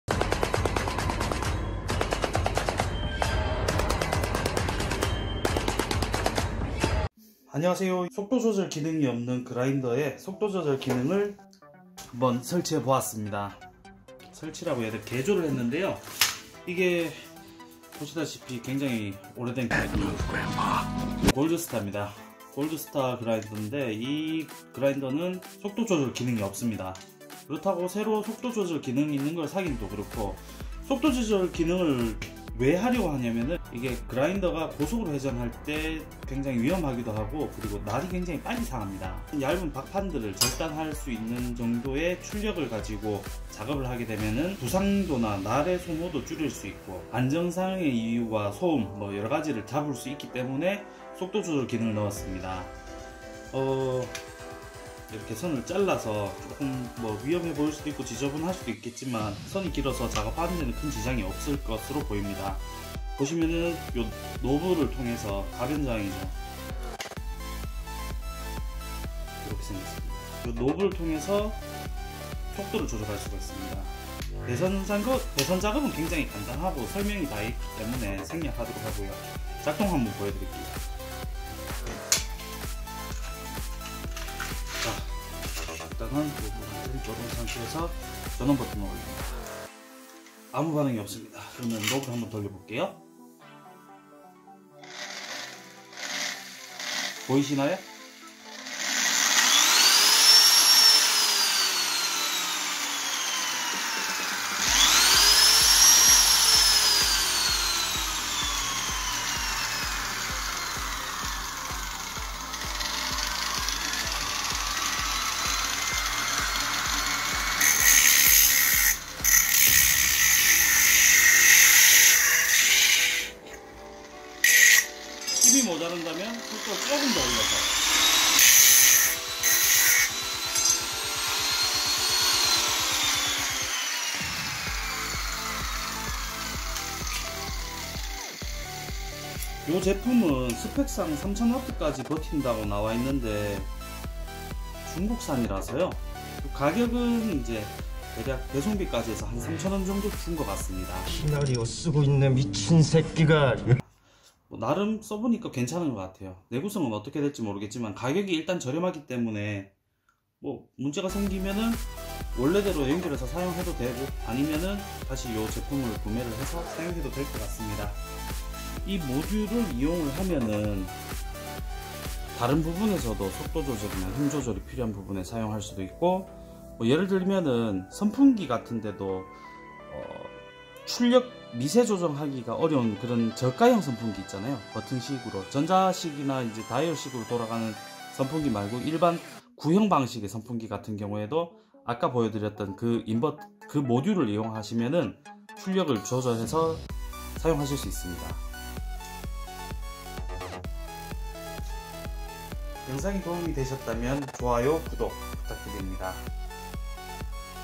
자작용 오래된 구형 그라인더에 속도조절 기능을 저렴하게 추가해보자.
이 영상은 전문적인 장비를 소개하는 것이 아닙니다.
 속도 조절이 되지 않는 장비에 저렴하게 장비 교체 없이 사용하도록 개조하는 것을 소개한 영상입니다.
오로지 취미용이고 개조한지 2년이 되어가지만 아직도 성능에 이상 없이 잘 사용하고 있습니다.
전문적인 고성능 장비를 생각하신분이라면 이번 영상은 도움되지 않을 것입니다.

골드스타... 죽지도 않고 오래가는 전설의 그라인더... 우수한 내구성으로 아직도 사용가능한 고마운 녀석입니다.
요즘 절단작업을 부쩍 많이 하다보니.. 그라인더 작업중 아찍했던적이 한부번이 아니었습니다. 이러다 손구락 하나 해먹을까 싶어 절단이 되는 최소한의 속도로 작동하게끔 그나마 좀 안전하고 조용하게 사용하고 싶어 개조를 해보았습니다. 외형은 험하지만 정상작동하고 있는 그라인더가 있기에 비싼 돈들여 새로 그라인더를 구매하는 것은 바람직하지 못다하고 생각해서
중국에서 저렴하게 레귤레이터를 구매합니다. 사용법은 간단해요
그라인더 전원코드에서 약 200mm정도 위치에서 잘라 표시된 AC입력부 출력부에 각각 전선을 연결해주기만 하면 됩니다.
이걸 응용하면 속도 미세조정이 필요한 전동기부문에는 전부 적용가능해 보입니다.
선풍기를 예로 들자면 여름에 기분좋은 만큼의 저회전이 가능했으면 좋겠다는 생각이 들때가 있습니다. 비싼 선풍기는 순풍 미풍 수면풍등의 이름으로 불리지만 저렴한 사은품 선풍기능은 1단 2단  밖에 없죠... 1단은 약하고 2단은 너무 세거나 1단보다 조금더 약한 바람이 필요할때 이 레귤레이터를 장착하면 됩니다. 표시는 4000W라 하지만 저는 3000W정도로 보고 사용햇습니다.
어느정도 마진을 둬야 그나마 오래 사용할것 같아서요..